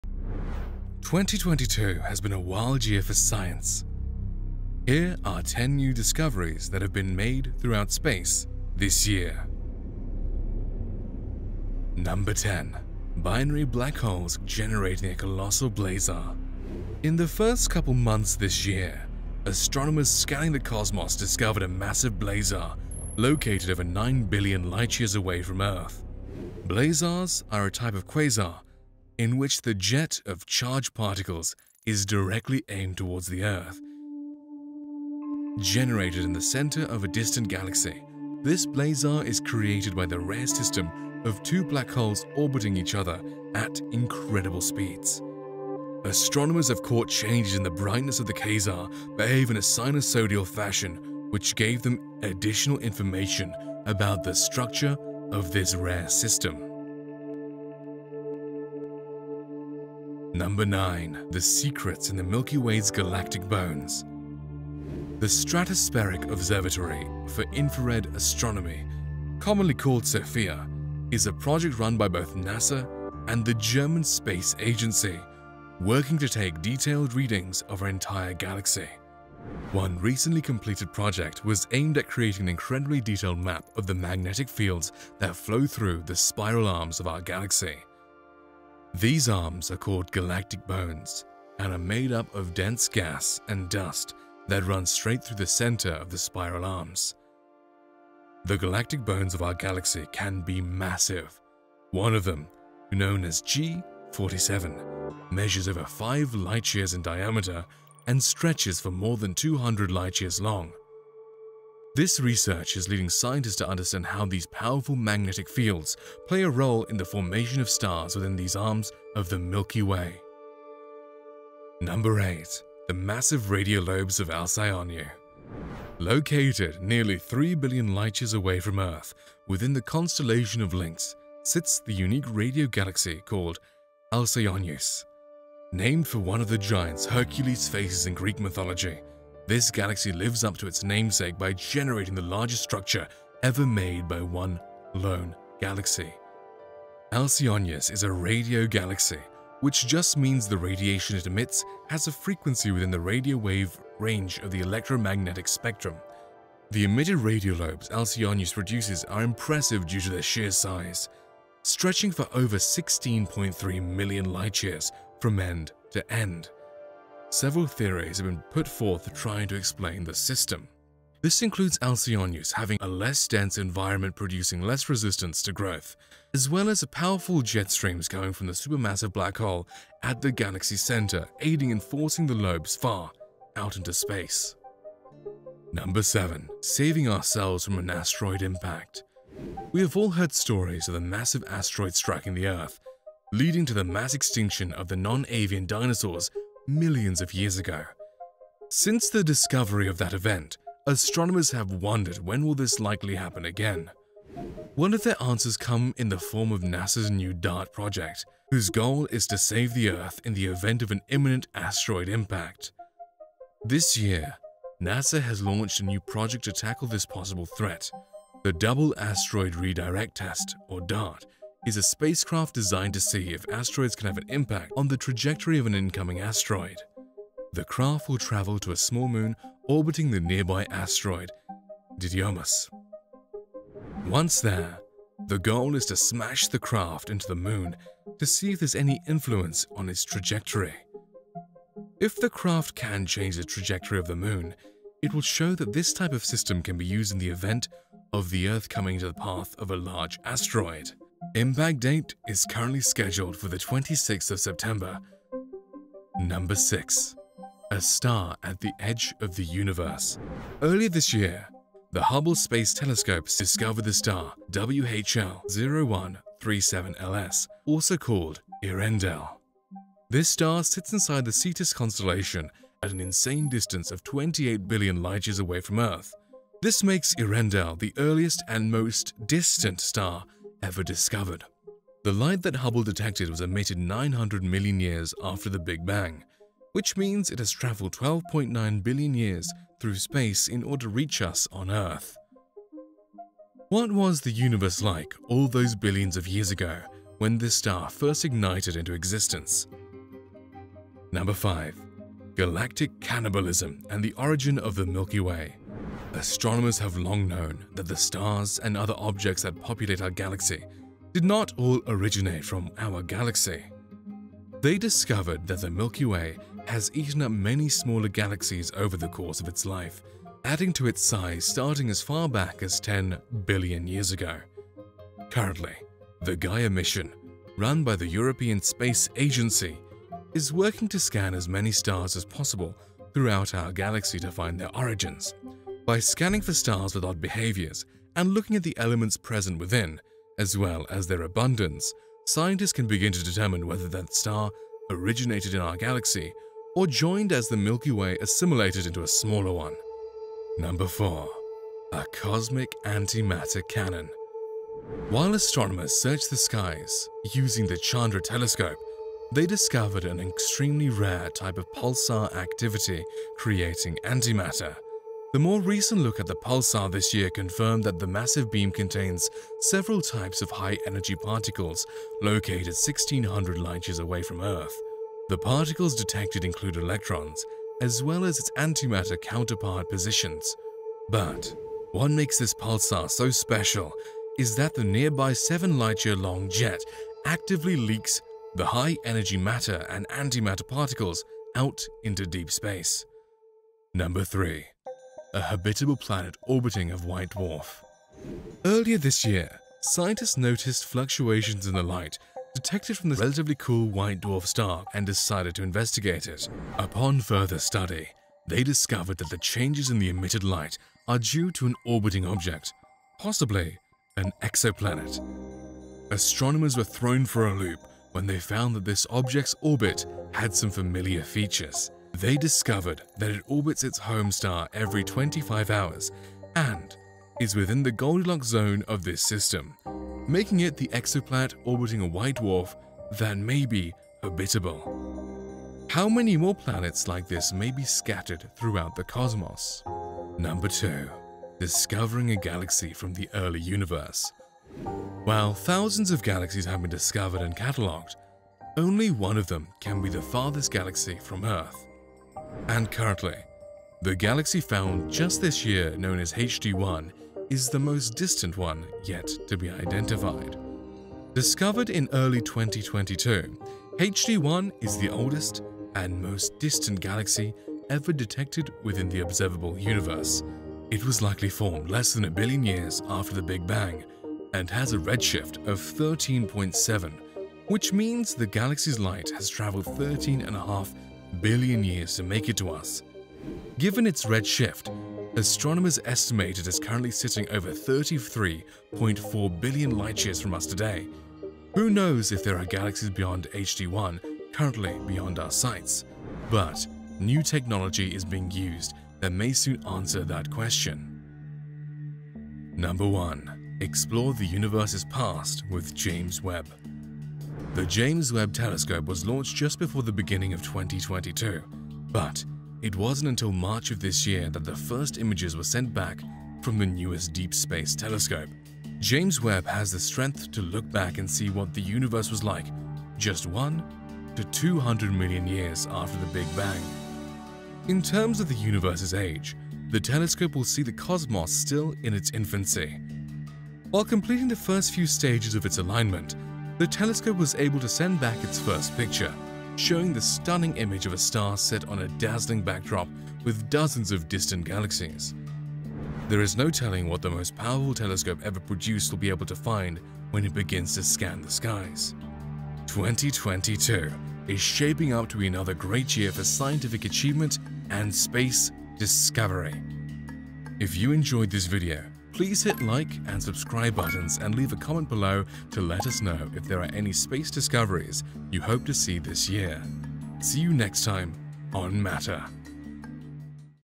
10 INSANE Space Discoveries 2022
10 INSANE Space discoveries 2022! In 2022, some of the most amazing discoveries have been made, especially when it comes to space and the Universe. What amazing space discoveries have been made in 2022? Will these 2022 space discoveries change the way you see our Universe?

Topics Discussed/Related:
- 10 Space Discoveries 2022
- Space Discoveries made in 2022
- Incredible space discoveries